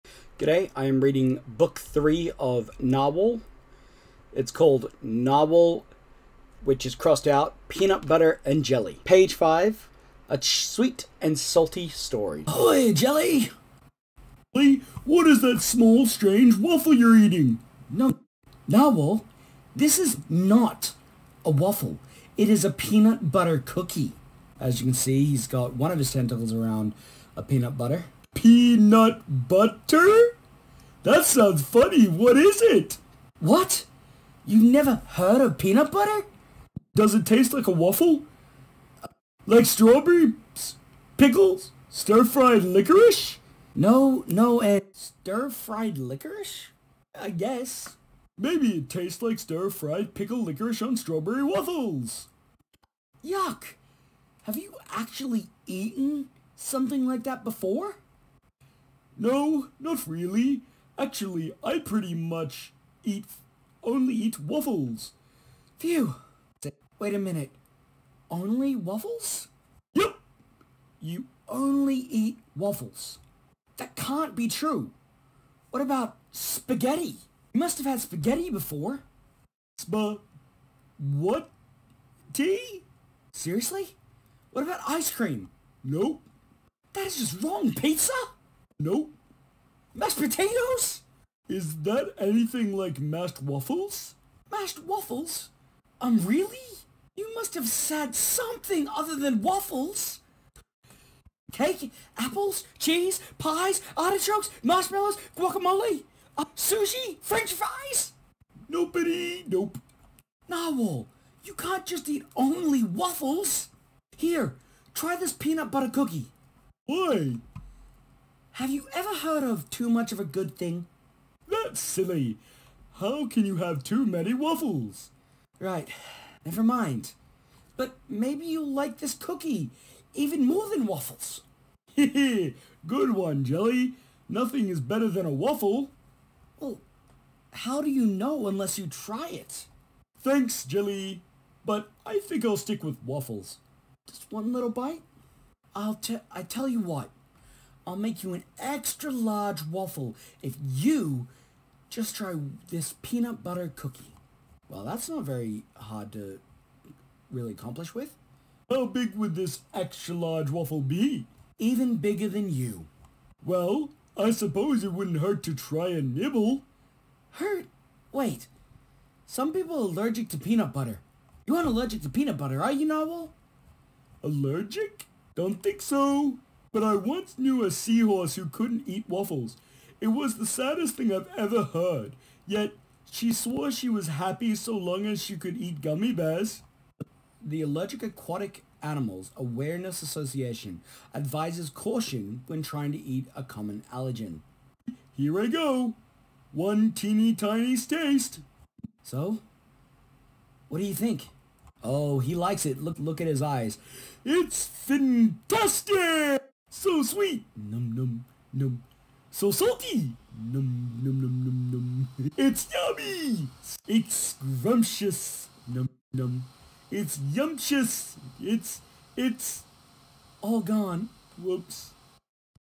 Narwhal Peanut Butter & Jelly - A Sweet & Salty Story| Read By Isaac Letchford
In this video, by son reads the first section of Narwhal and Jelly, a popular entry of the Narwhal series of visual novels for children.

 This section of the book introduces the two main characters of the series, Narwhal and Jelly! If this sounds exciting to you, please tune in and look forward to the next few parts of this hilarious and charming series of chapters.